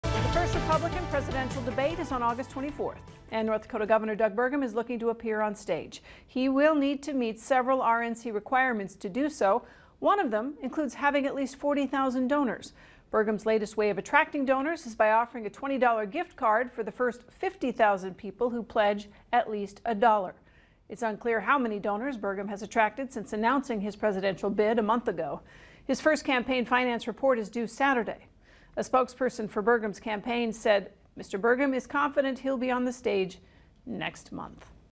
North Dakota Gov. Doug Burgum offering $20 gift cards as presidential candidate seeks 40,000 donors
Gov. Doug Burgum offering $20 gift cards as presidential candidate seeks 40,000 donors